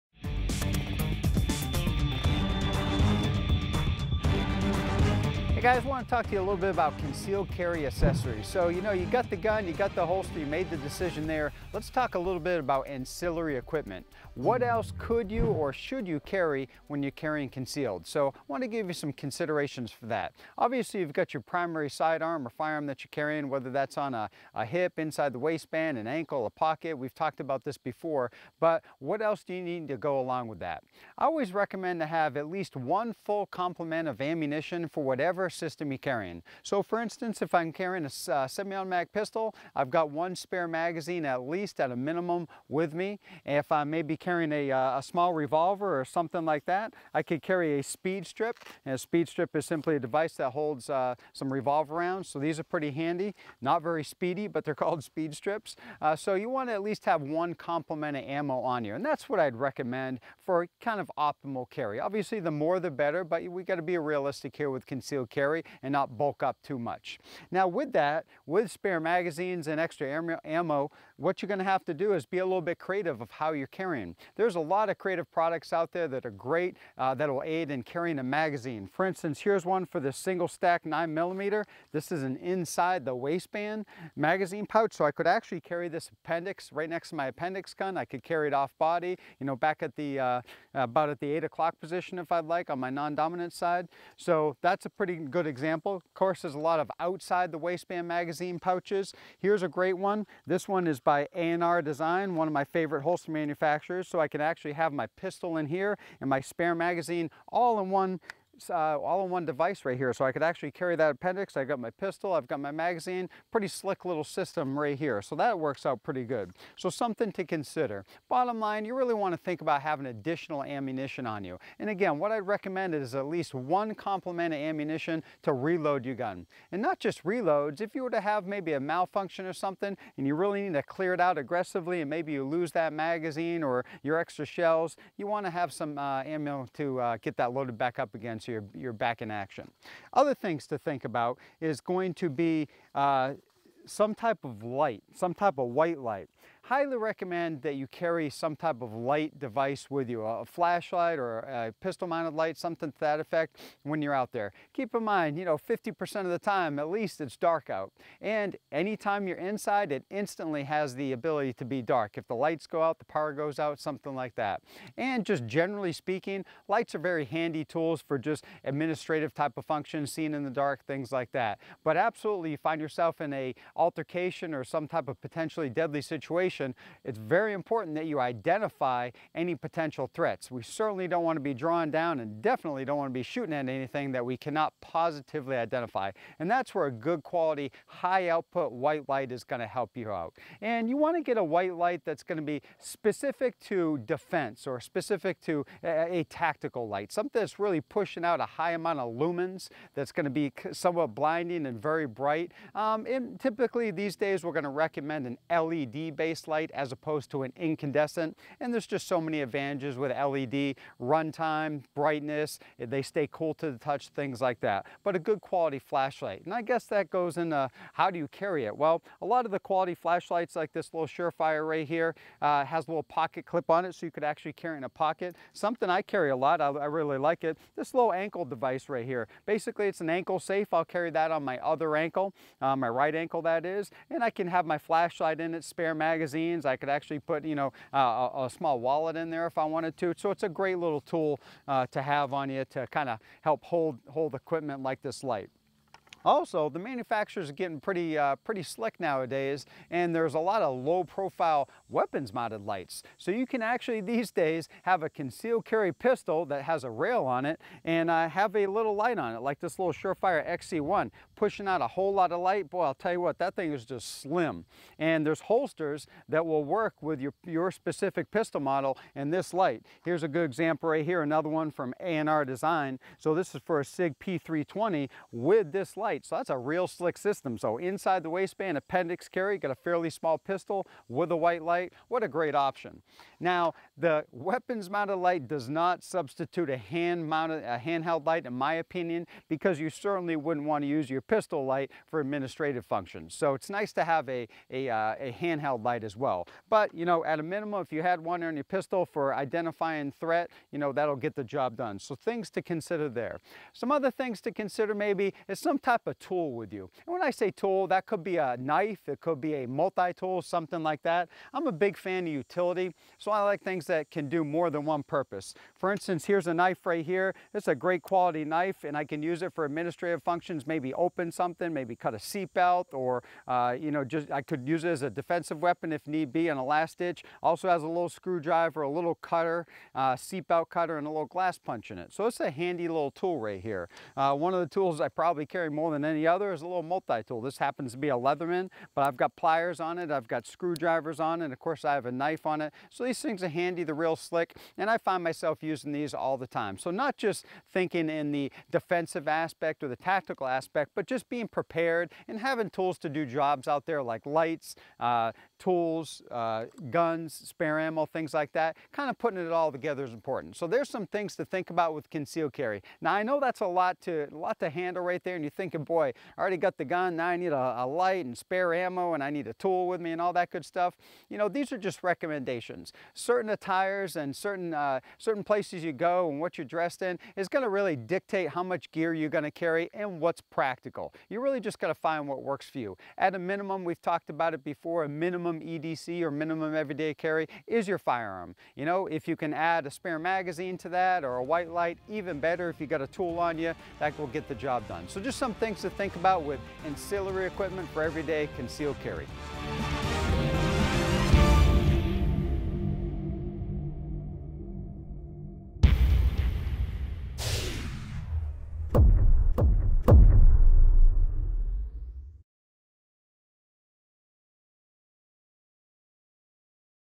Accessories | E3 Firearms Conceal Carry Basics
Welcome back to Part Two of the E3 Firearms Concealed Carry Basics video series - this time, delving into the realm of firearm accessories! Join our Co-Founder and Master Firearms Instructor, Adam Painchaud, as he guides you through the essential accessories that can truly enhance your concealed carry experience.

Embark on a deep dive into the world of concealed carry accessories, exploring must-haves that optimize your setup. From holsters and belts to magazine carriers and cutting-edge sighting systems, Adam Painchaud shares expert insights to help you make well-informed decisions tailored to your personal preferences.

Uncover the latest innovations and practical tips for selecting accessories that not only maximize comfort but also significantly contribute to the overall effectiveness of your concealed carry strategy. Our comprehensive coverage guarantees that you'll be equipped with the knowledge to make informed choices, aligning seamlessly with your individual lifestyle.

Take your firearms education to new heights, covering diverse areas such as defensive pistol tactics, firearm care, and maintenance.

Don't miss out on the chance to elevate your concealed carry skills.

today to sign up and empower yourself with the essential information for responsible and safe concealed carry.